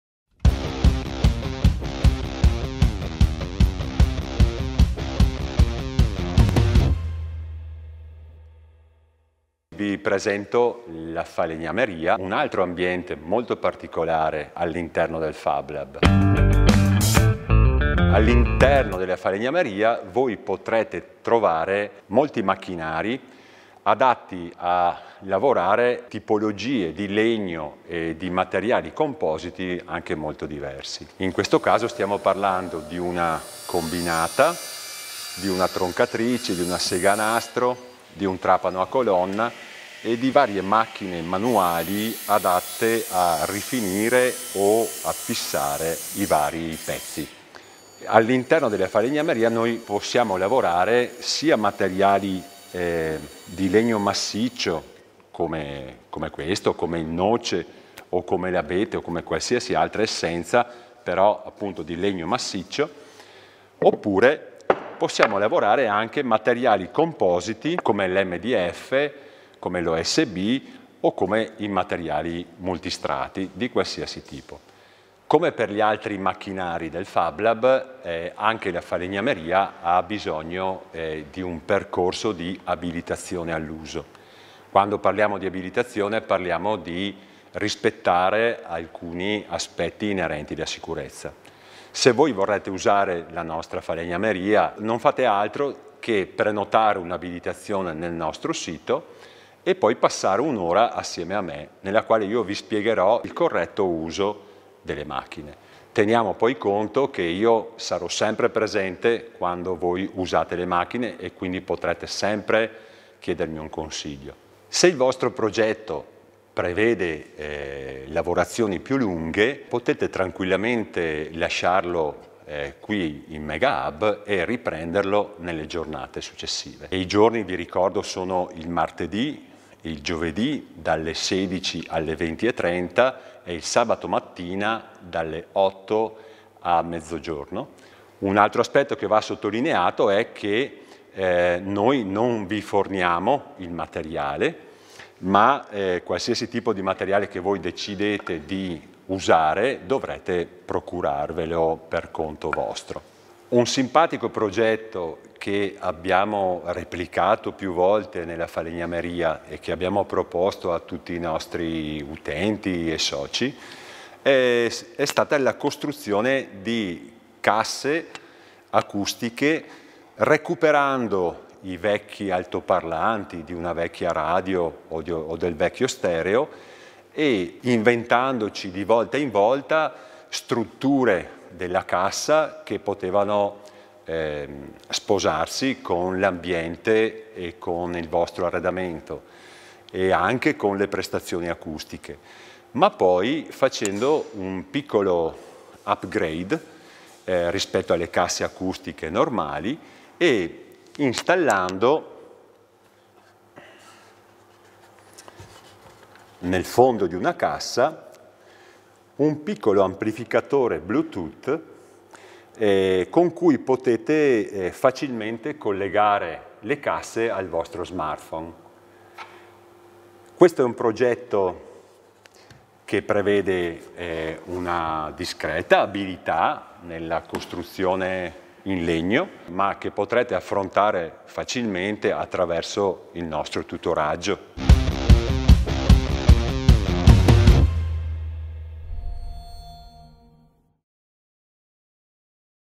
La falegnameria di Megahub e le casse acustiche autoprodotte
Giuseppe, tutor della falegnameria di Megahub, spiega cosa potete fare nella nostra officina riguardo la lavorazione del legno e mostra  le casse acustiche autoprodotte.